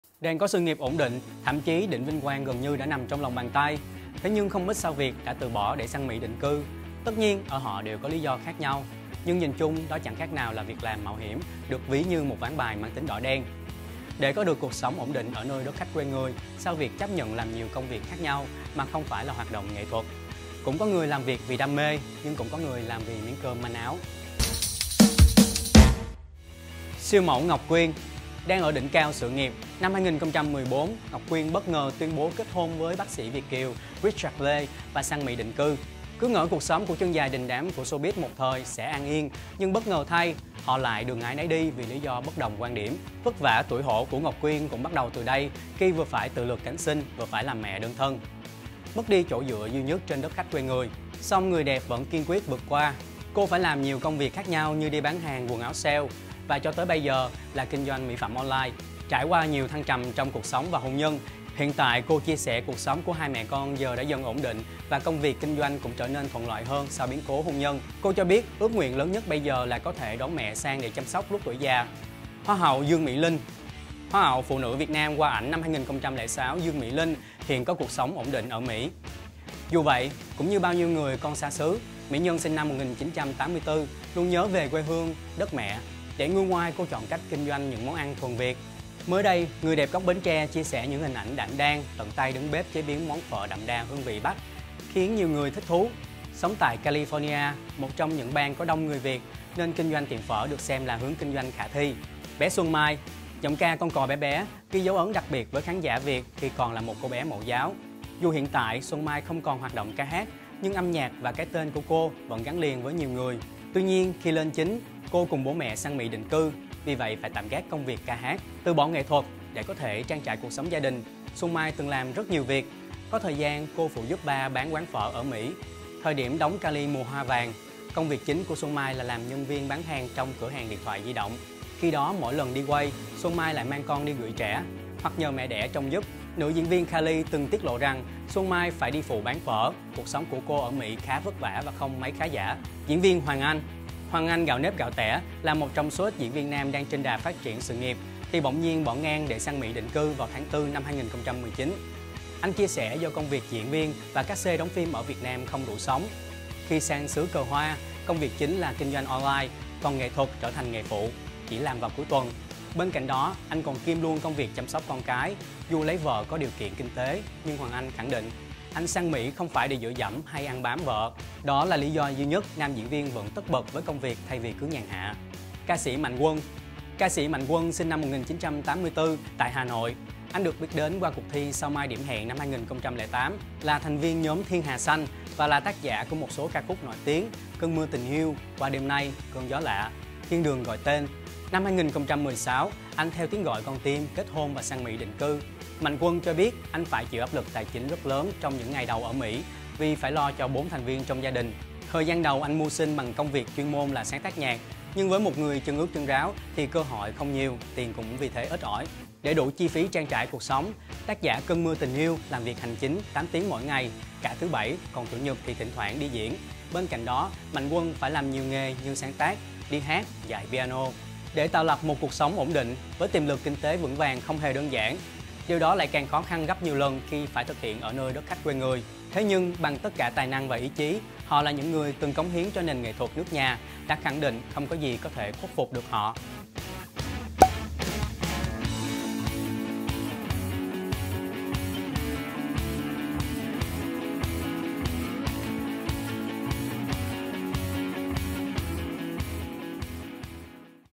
Đang Có Sự Nghiệp Ổn Định, Nhưng Không Ít Sao Việt Đã Từ Bỏ Để Sang Mỹ Định Cư.
Hãy theo dõi Yan trên:
✔️Youtube Yan News :